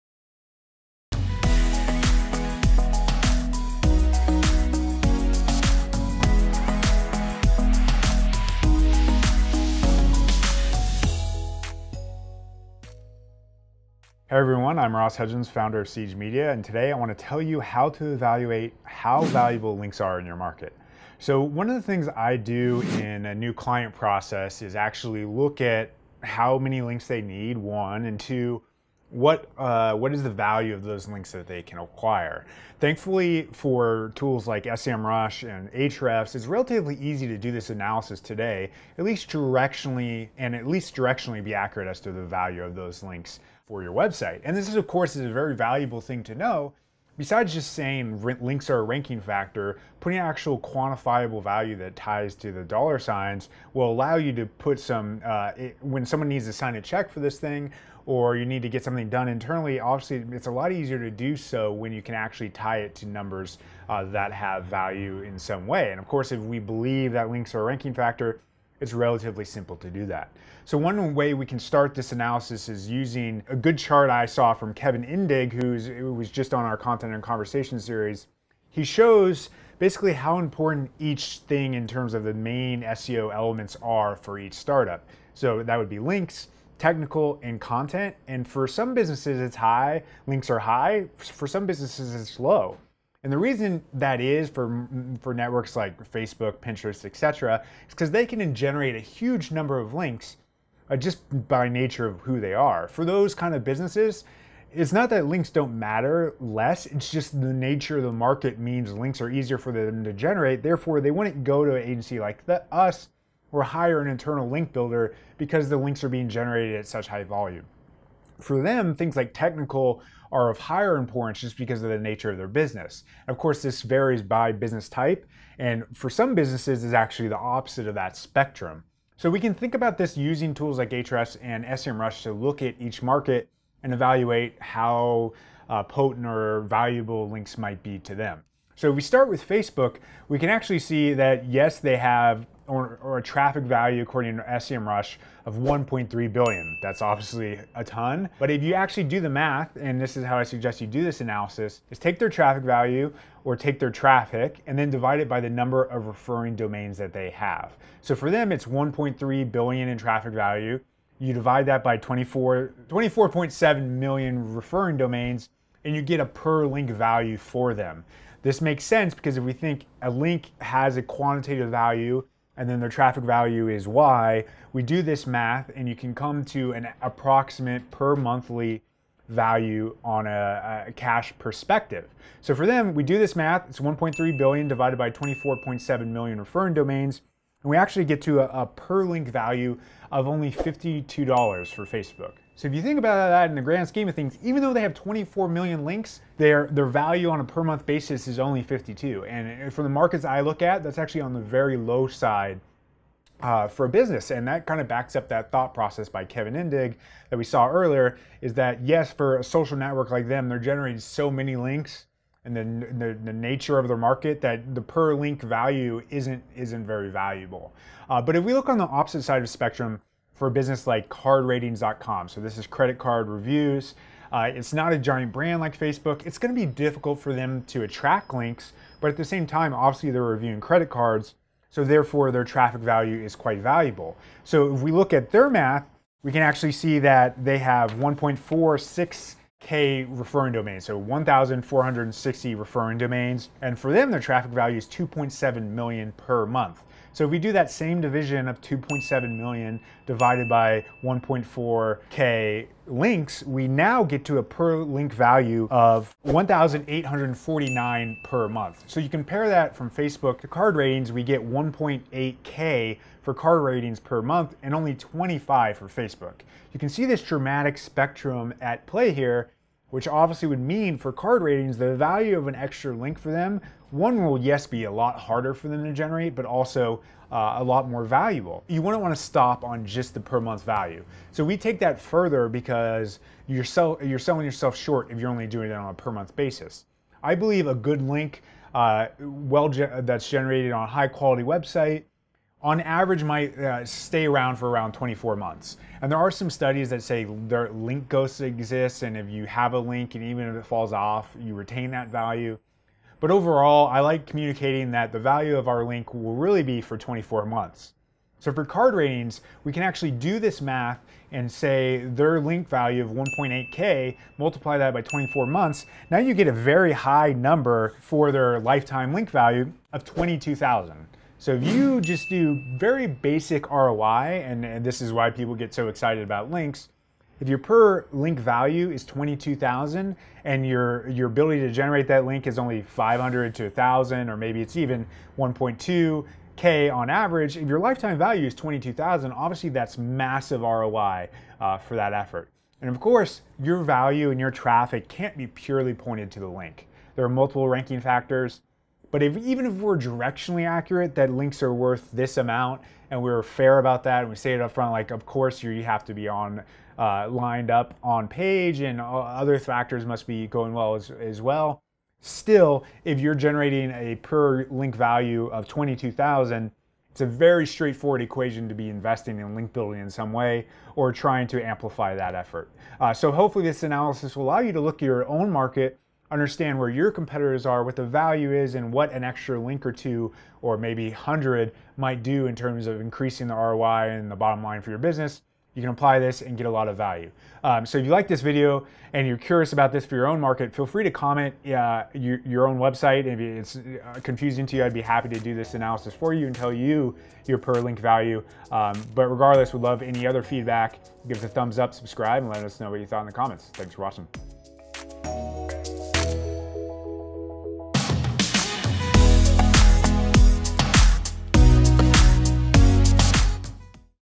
How Valuable Are Links in Your Market?
Every industry values links a little differently. Thanks to great tools like Ahrefs and SEMRush that exist today, we can actually apply dollar signs to the value of links to determine if link building makes sense for us, and also get buy-in for our content marketing projects. In today's video, Ross shares the content marketing formula we use for new clients as well as existing ones to prove the value links have.

NOTE: We use the phrase “links” in some of these equations, but when we do we mean linking root domains, not links in total given diminishing returns from links from the same domain.